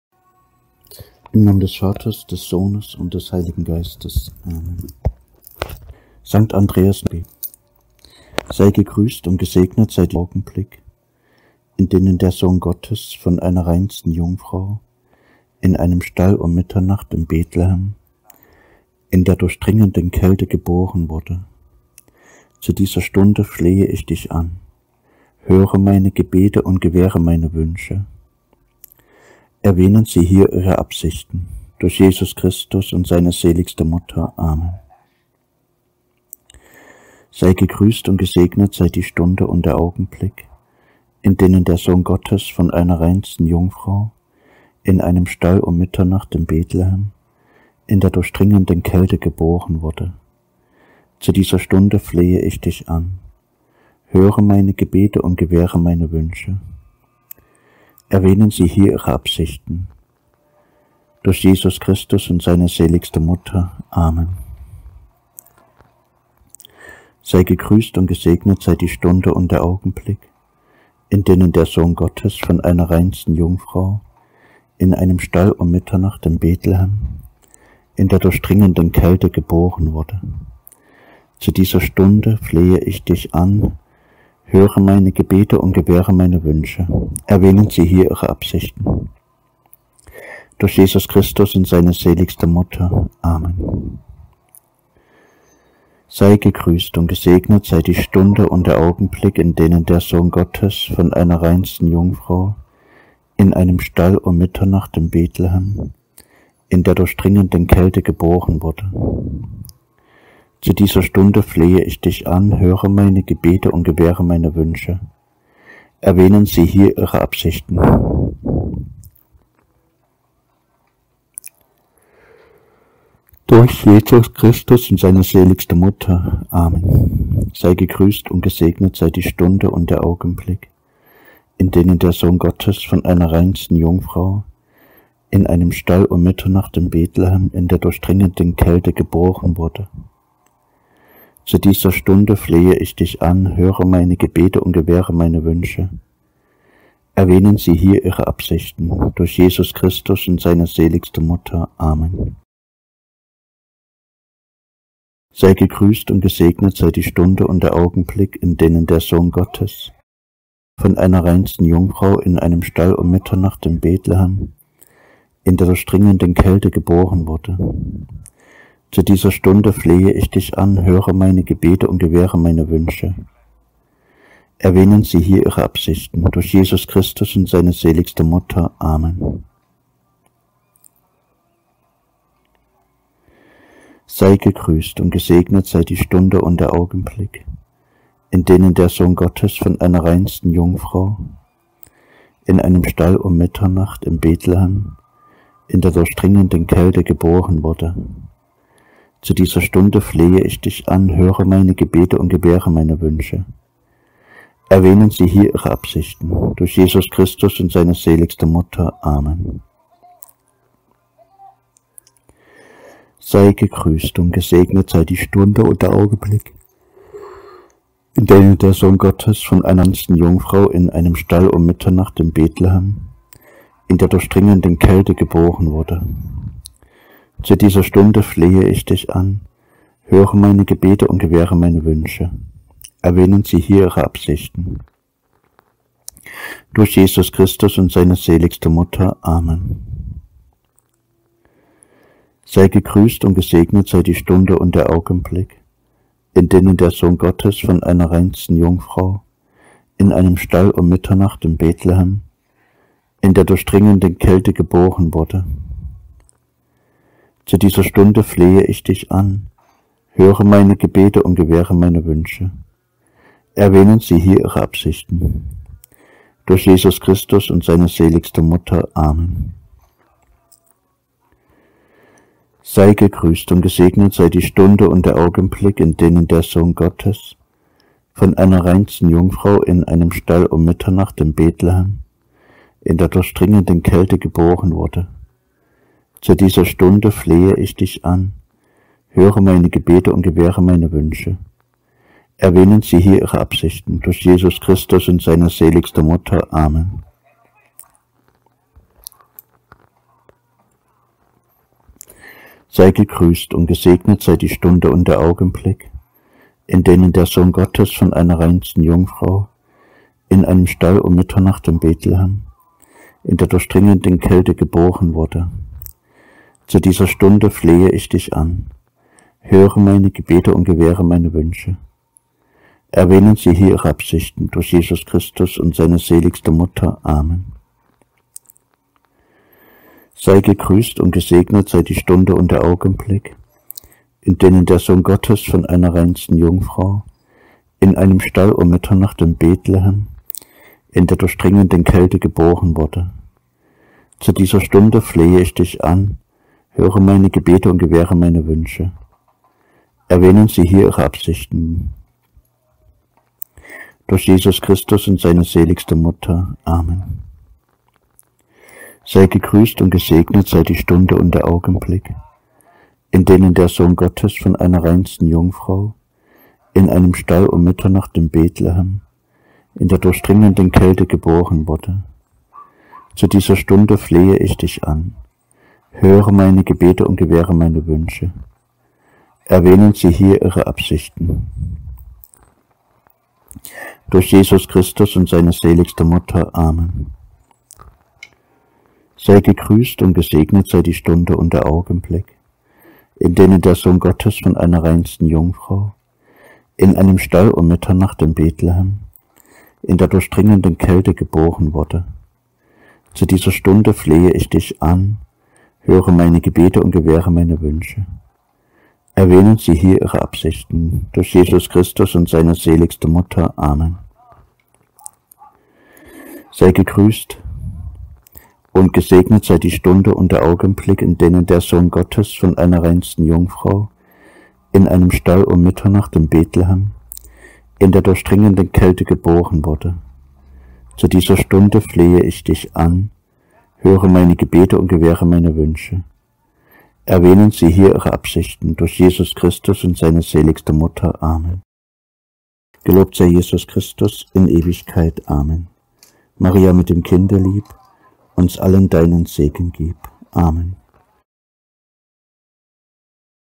18/24 St. Andreas Novene Gebet
St. Andreas Novene Gebet :

Sei gegrüßt, und gesegnet sei die Stunde und der Augenblick, in denen der Sohn Gottes von einer reinsten Jungfrau in einem Stall um Mitternacht in Bethlehem in der durchdringenden Kälte geboren wurde. Zu dieser Stunde flehe ich dich an, höre meine Gebete und gewähre meine Wünsche .

(Erwähnen Sie hier Ihre Absichten)
Durch Jesus Christus und seine seligste Mutter.
Amen .

Traditionell wird dieses Gebet 15 Mal am Tag für die Dauer der Novene gebetet.